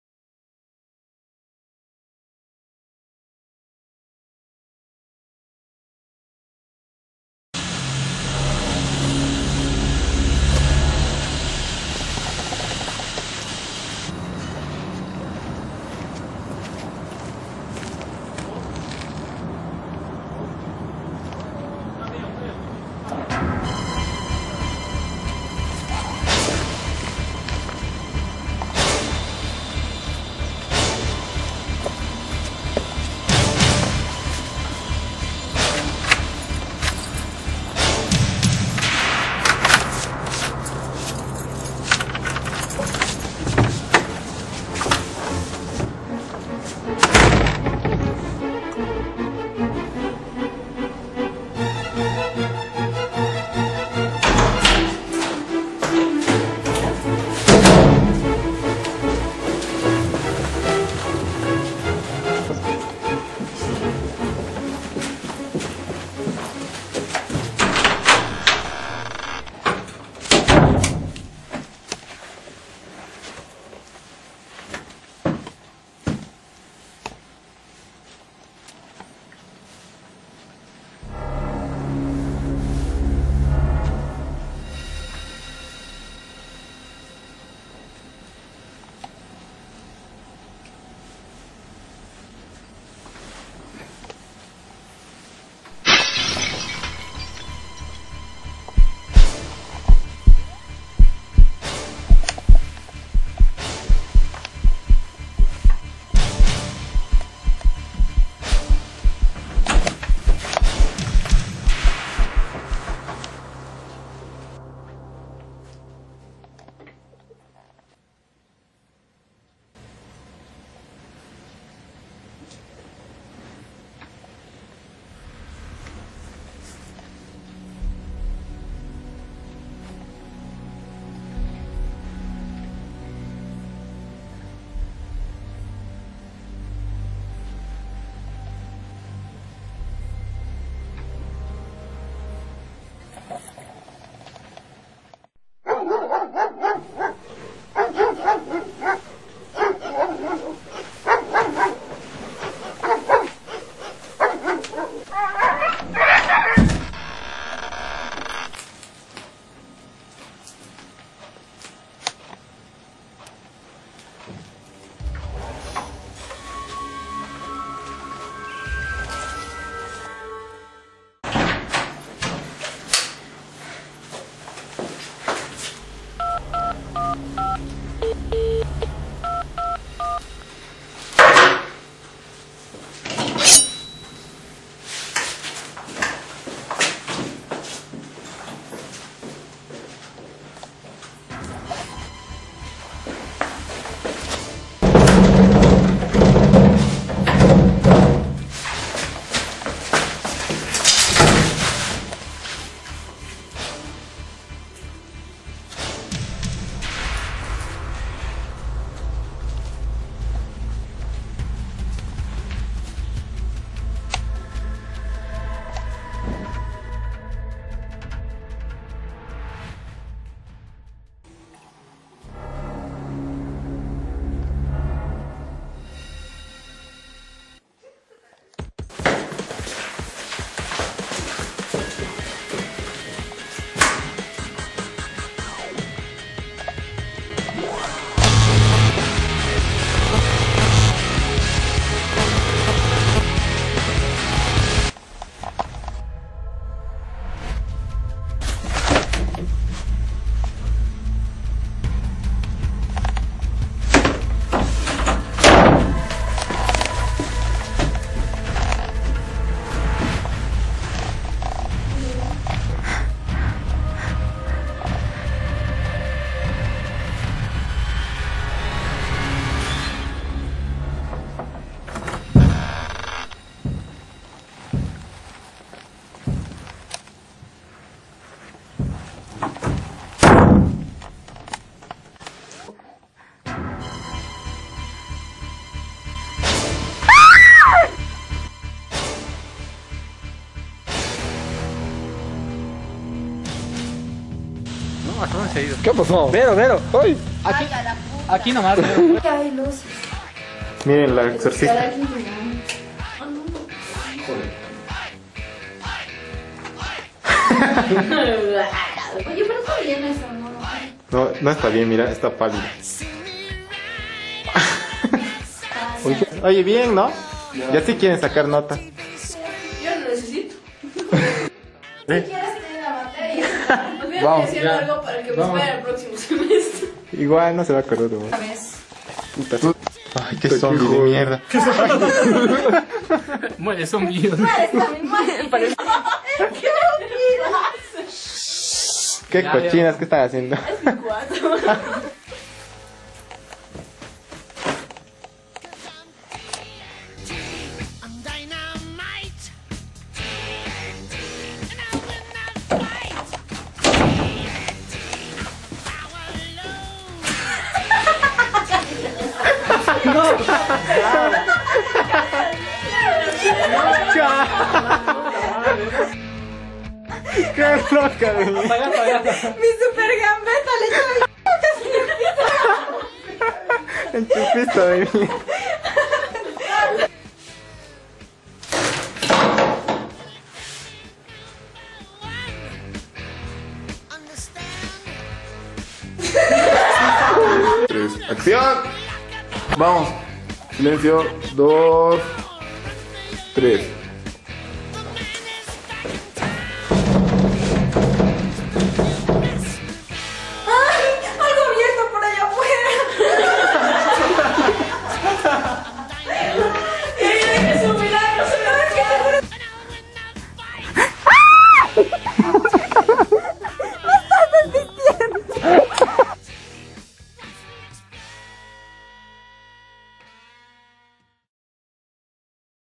Depredador Nocturno
Cortometraje de audio con temática terrorífica y bastante suspenso (Excelentes bloopers sobre cómo se realizó el corto)